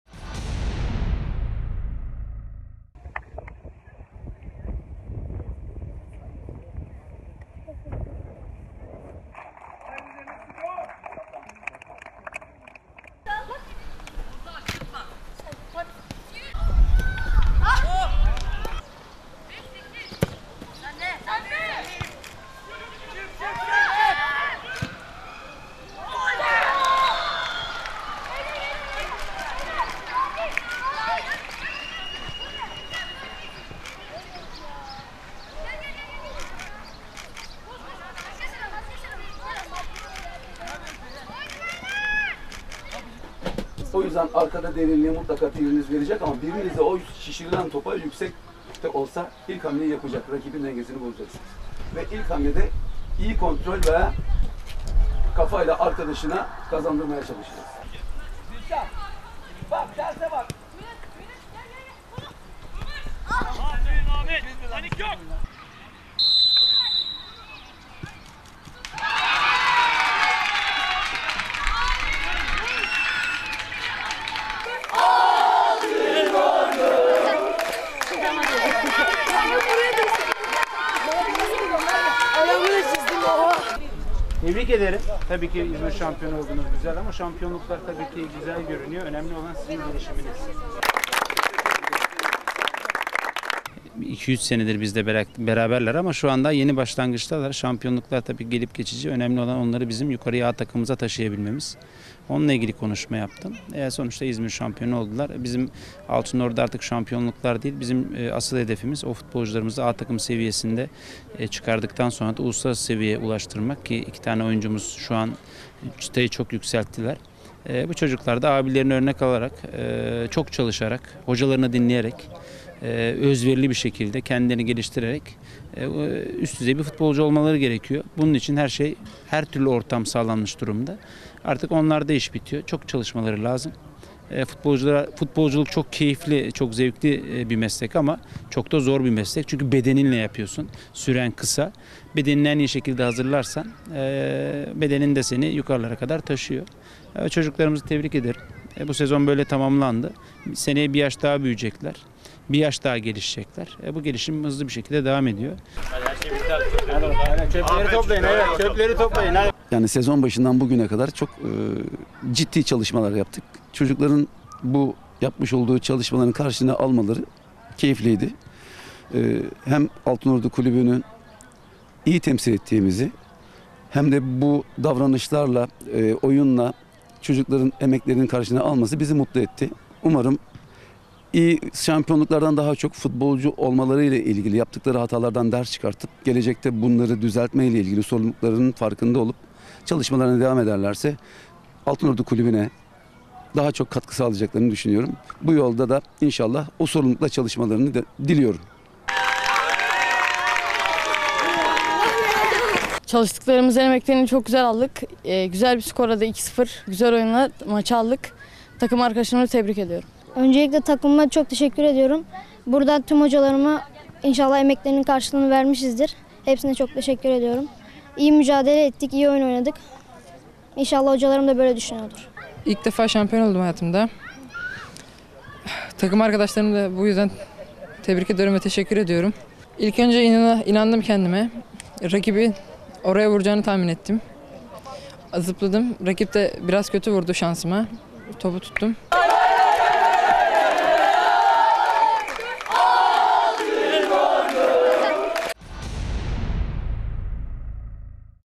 ALKIŞLAR U13'LERİMİZE
İZMİR ASKF Final Grubu son maçında Denizspor'u 2-0 yenen çocuklarımız şampiyon oldu.